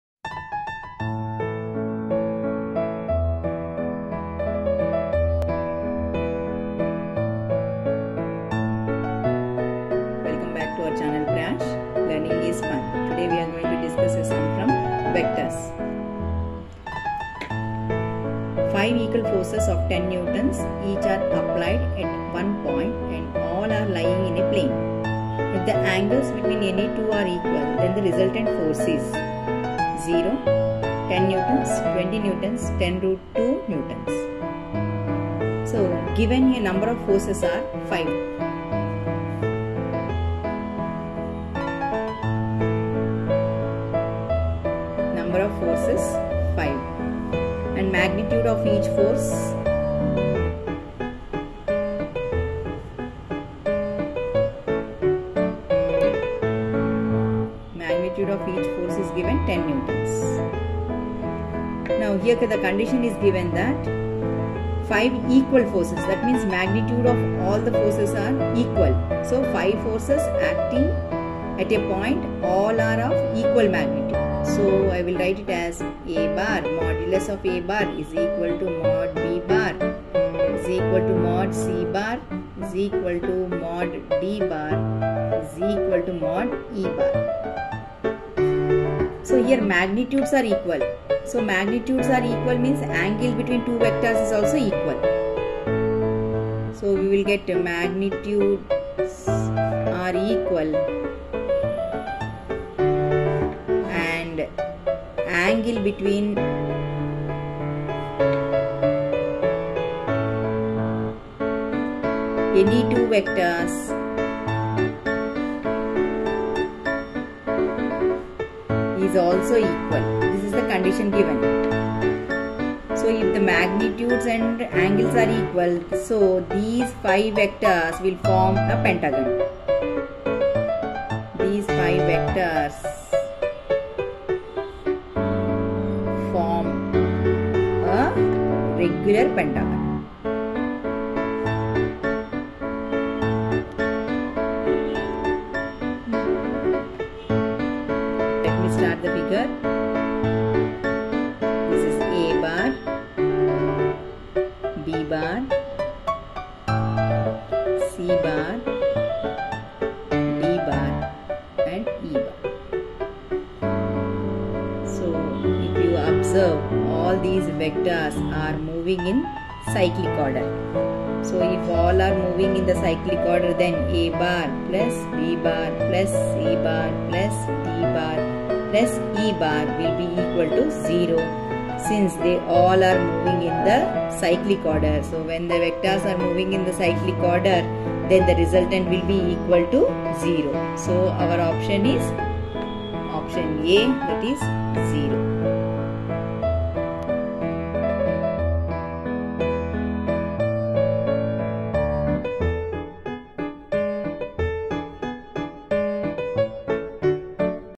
Five equal forces of 10N each are applied at one point and all are lying In a plane. || PRASHY
Five equal forces of 10N each are applied at one point and all are lying In a plane. If the angles between any two are equal the resultant  force is

|| VECTORS || PHYSICS || PRASHY ||